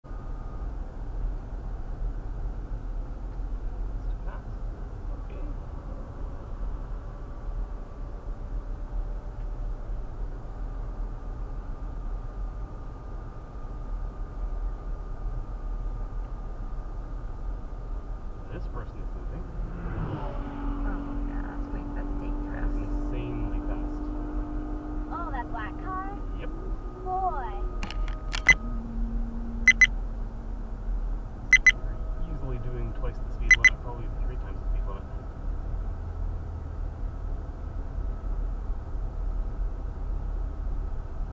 Nissan passed pretty quickly in Port Alberni (260 km/h)
I was doing about the speed limit (70km/h), maybe a bit above.  The pickup's pass was pretty normal.  The Nissan was beyond reason.

At 20.3s, the Nissan passes a power pole, and crosses the next power pole at 21.5s, taking 1.2s to travel that distance.  I passed that same set of poles at 21.0 and 25.5s, taking 4.5s to travel the same distance.  This means the Nissan was moving at 3.75x my speed, which is about 260 km/h.